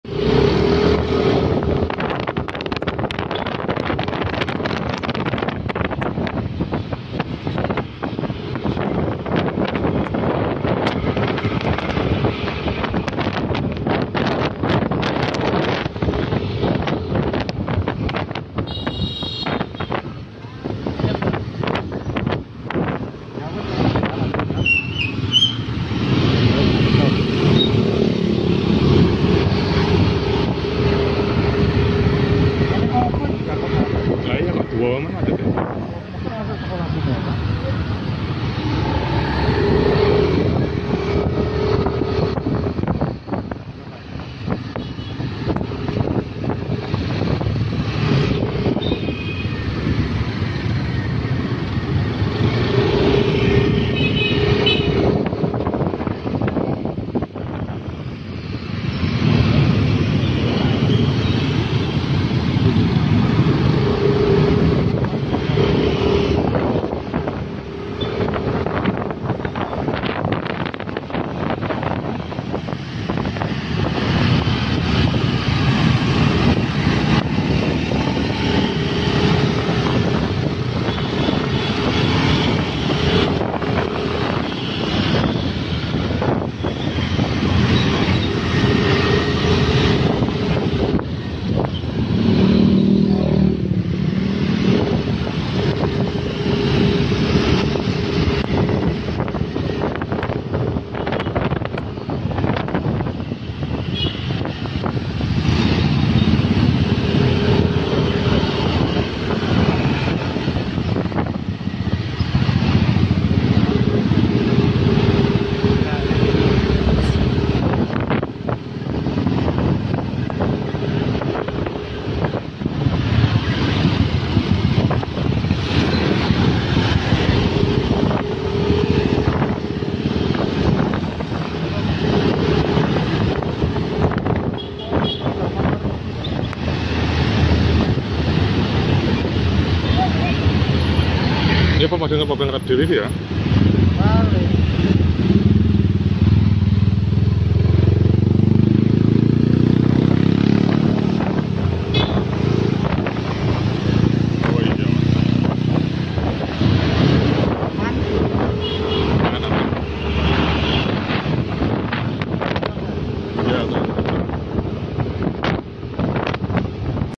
macet gara gara truk besar
gara gara kendaraan besar didepan melambat akhirnya banyak kendaraan dibelakangnya ikut juga melambat sampai memanjang